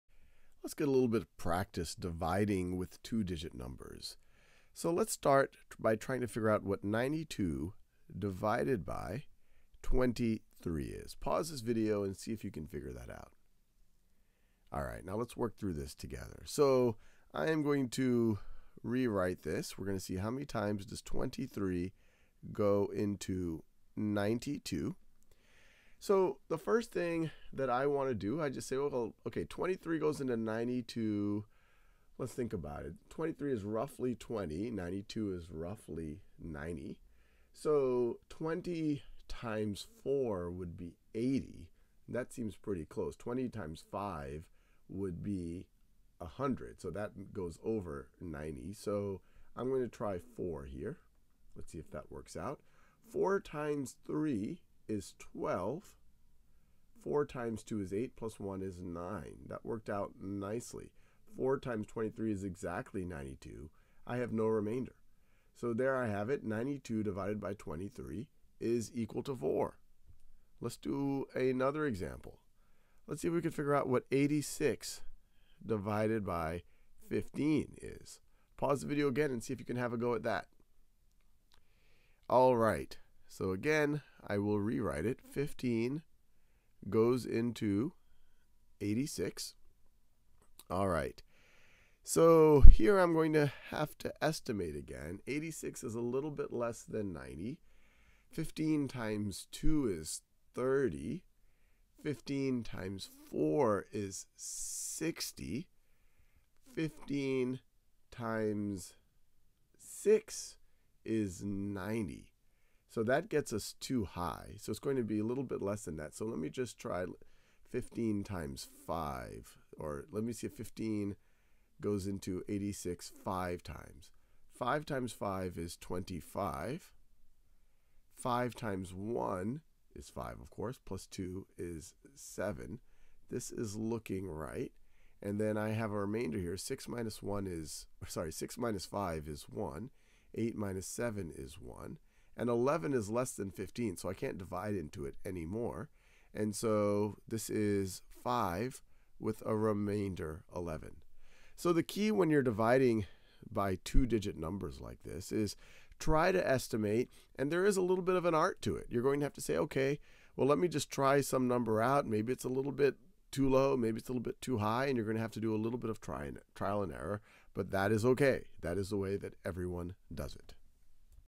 Dividing 2-digit numbers by 2 digit-numbers | Grade 5 (TX TEKS) | Khan Academy
Practice dividing 2-digit numbers by 2-digit numbers.